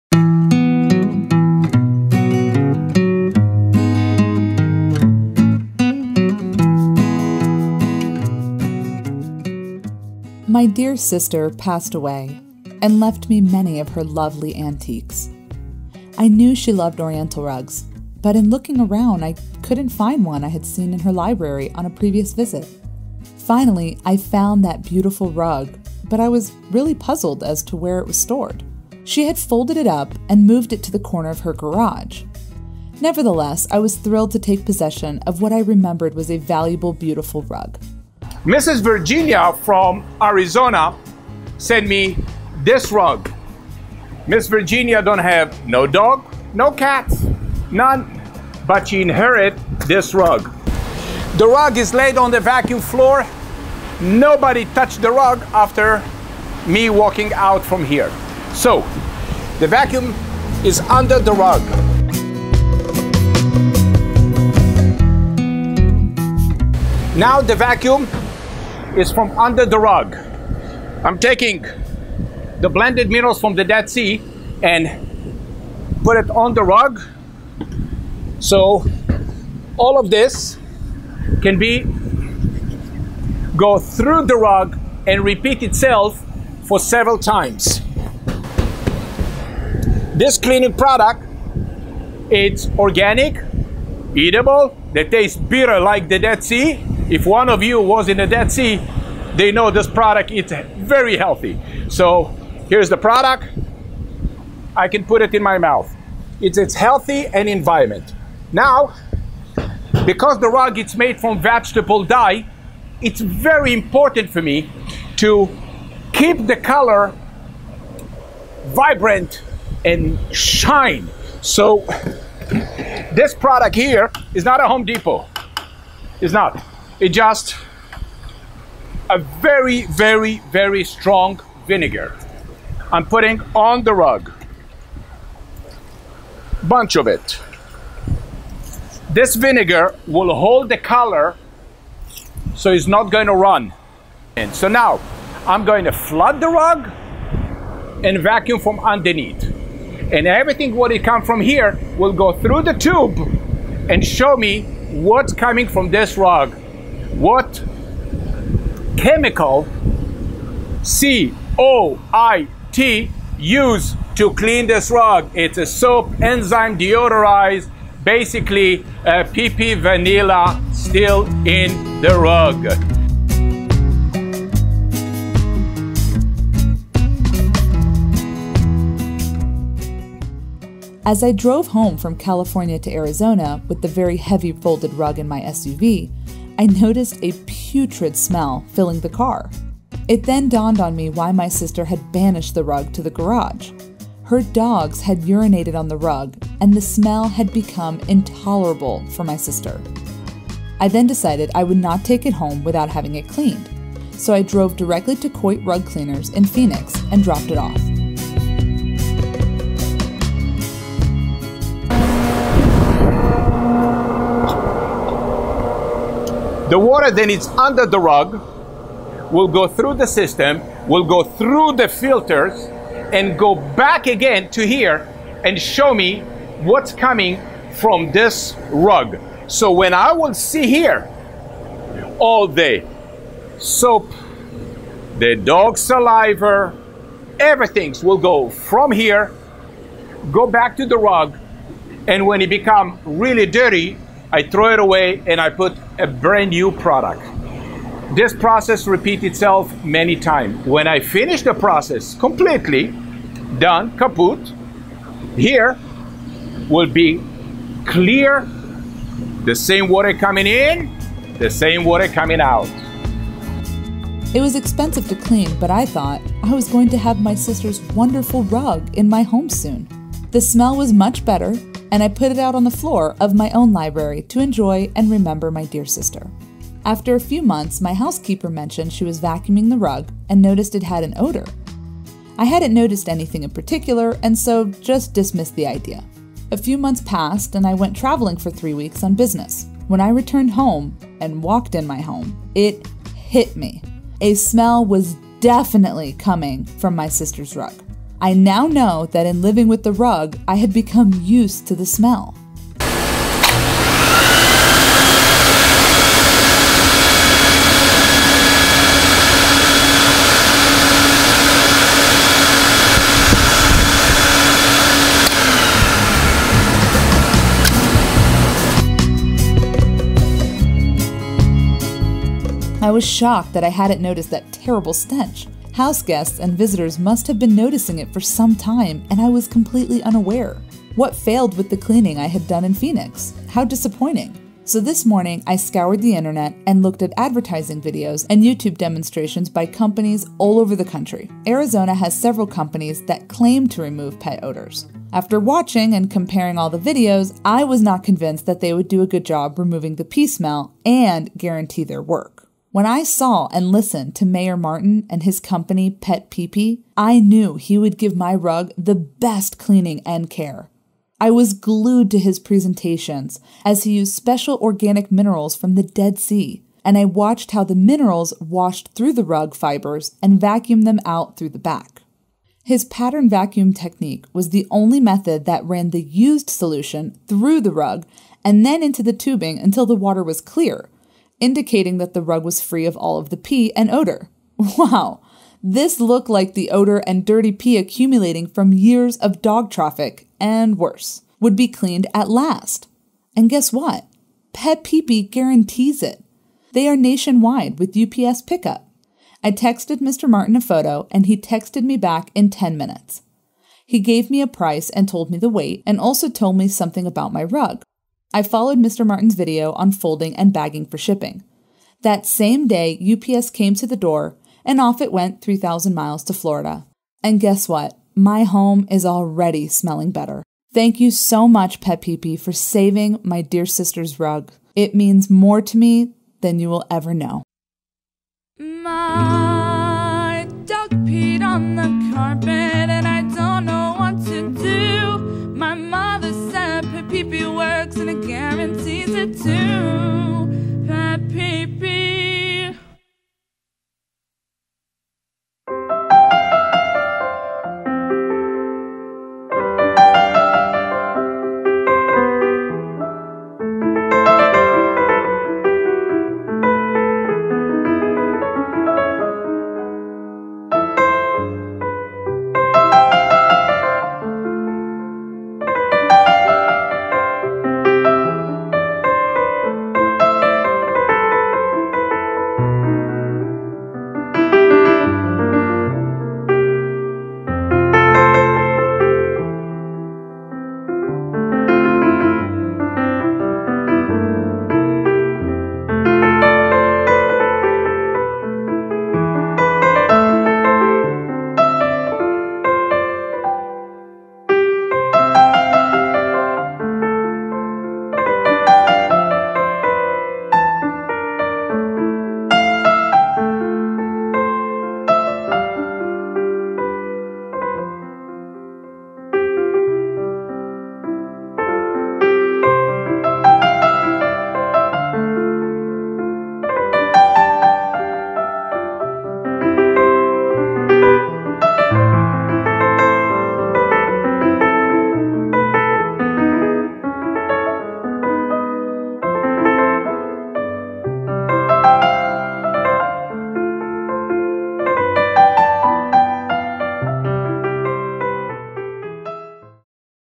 A TREASURE RUG THAT SMELLED BAD ORIENTAL RUG CLEANING
A TREASURED RUG THAT SMELLED BAD ORIENTAL RUG CLEANING

oriental rug cleaning from urine odor.

Visit Google PetPeePee Reviews

This Antique oriental rug is shipped from the State of Arizona.
To be Clean after the Colt carpet cleaner company cleaned the rug.
What Virginia wrote to me.

A treasured rug that smelled bad

Virginia H
to me
My dear sister passed away and left me many of her lovely antiques.
I knew she loved Oriental rugs, but I couldn't find one I had seen in her library on a previous visit in looking around.

Finally, I found the beautiful rug, but I was very puzzled about where it was stored. She had folded it up and moved it to the corner of her garage.

Nevertheless, I was thrilled to take possession of what I remembered was a valuable, beautiful rug.

As I drove home from California to Arizona with the heavy folded rug in my SUV, I noticed a putrid smell filling the car.

I realized why my sister had banished the rug to the garage. Her dogs have urinated on the carpet, and the smell has become intolerable for my sister.

I then decided I would not take it home without cleaning it, so I drove directly to Coit rug cleaners in Phoenix and dropped it off.

It was expensive to clean, but I thought I would have my sisters a fabulous rug in my home soon.

The smell was much better, and I put it on the floor of my own library to enjoy and remember my dear sister.

After a few months, my housekeeper mentioned she was vacuuming the rug and noticed it had an odor.
I hadn't noticed anything in particular, so I dismissed the idea.

A few months passed, and I traveled for three weeks on business. When I returned and walked into my home, it "hit" me. The smell was definitely coming from my sister's rug.

I now know that I had become used to the smell of living with the rug. I was shocked that I hadn't noticed that terrible stench.
Houseguests and visitors must have noticed it for some time, and I was unaware.

What failed with the cleaning I had done in Phoenix? How disappointing.

So, this morning I scoured the internet and looked at advertising videos and YouTube demonstrations by companies all over the Country.
Arizona has several companies that claim to remove pet odors. After watching and comparing all the videos, I was not convinced they would do a good job removing the pee smell and guaranteeing their work.

When  I saw and listened to Meir Martin and his company Pet Pee Pee, I knew he would give my rug the best cleaning and care.

I was glued to his presentations as he used unique, organic minerals from The Dead Sea, and  I watched how the minerals washed through the rug fibers and vacuumed them out the back. His patterned vacuum technique was the only method that ran the used solution THROUGH the rug and then into the tubing until the water was clear, indicating the carpet was free of all the pee and odor.
Wow!!! This looked like the odor and dirty pee accumulating from years of dog traffic (and worse) would be cleaned.
And guess what? Pet Pee Pee guarantees it.

They are nationwide with UPS pick-up.
I texted Mr. Martin a photo, and he texted me back in ten minutes. He gave me a price, told me the weight, and told me something about my rug.

I followed Mr. Martin's video on folding and bagging for shipping. That same day UPS came to the door, and off it went 3,000 miles to Florida.
My home is already smelling better.